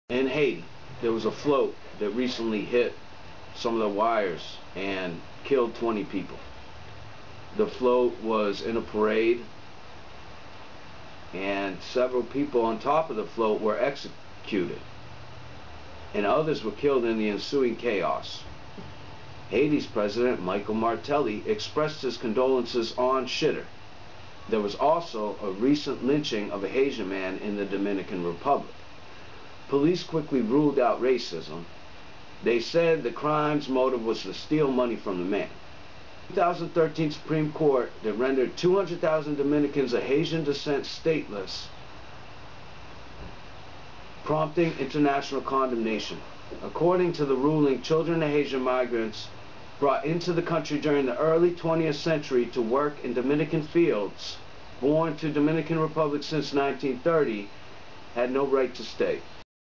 ProJect Haiti: Carnival Float Hits Power Line Killing 20+, Man Lynched for Money
Ras Reports *Head Line*